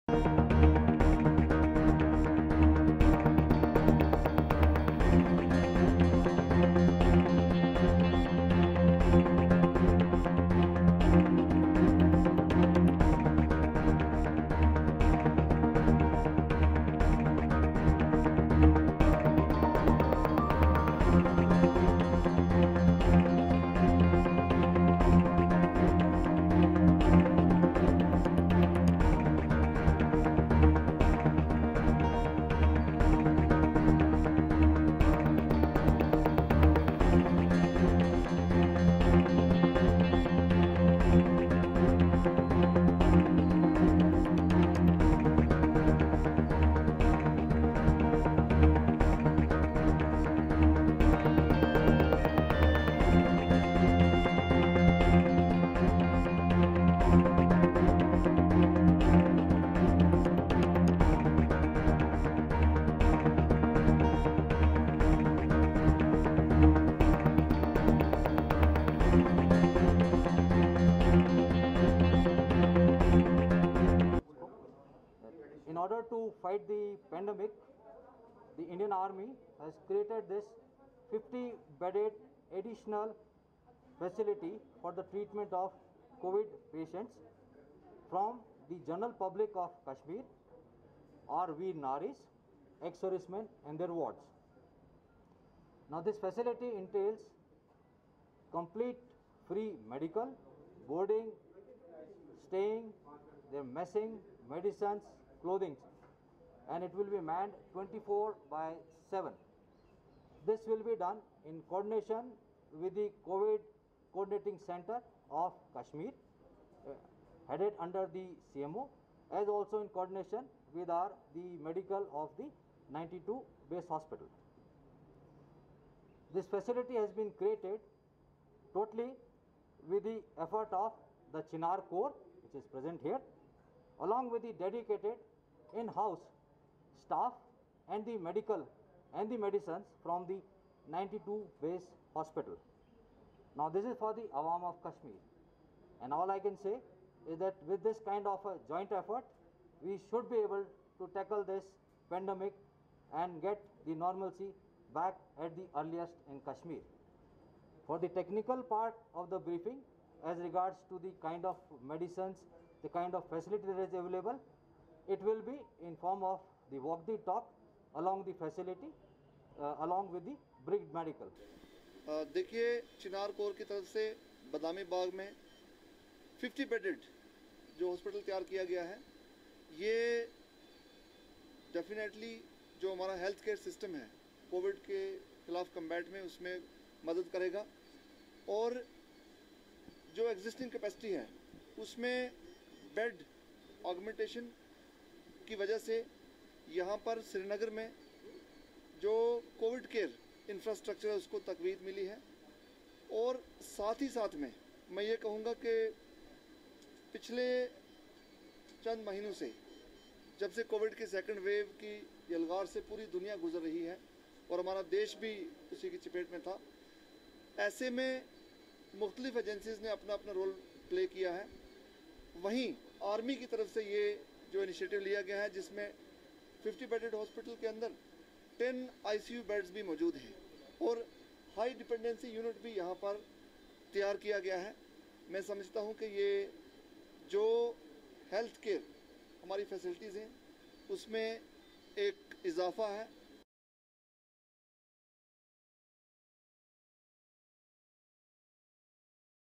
Army Dedicates 50 Bedded Covid Hospital For General Public
| Excelsior News |